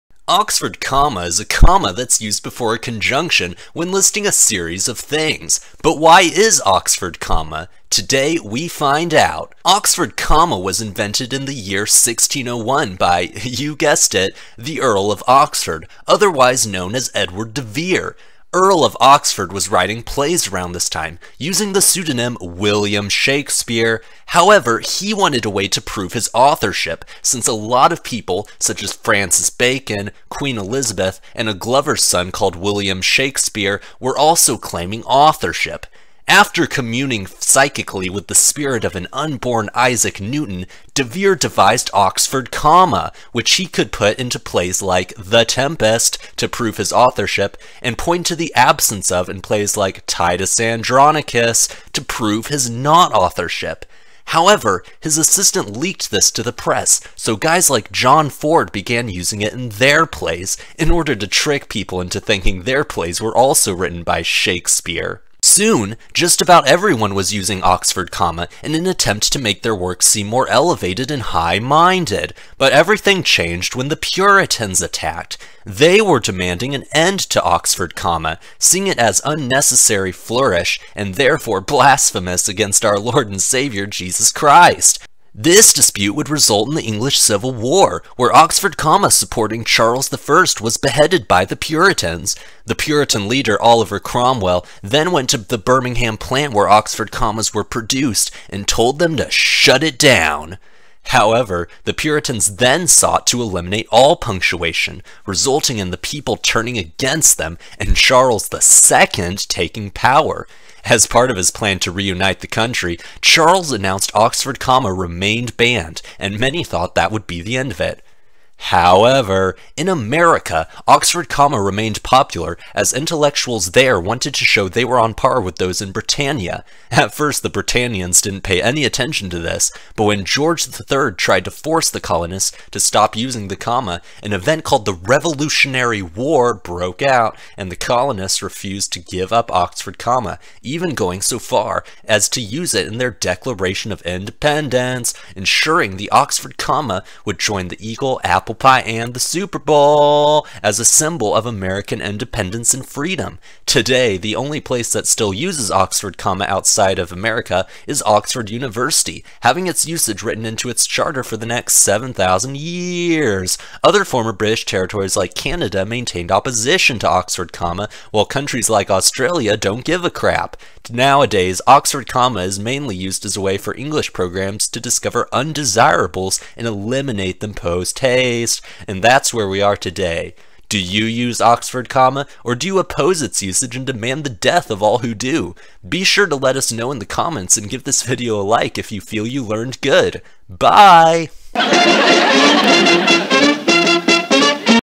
The History of the Oxford Comma
Everyone knows about the Oxford Comma, but did you know the exciting history behind it? In this video, we look at the  background and impact this piece of punctuation has made on our world.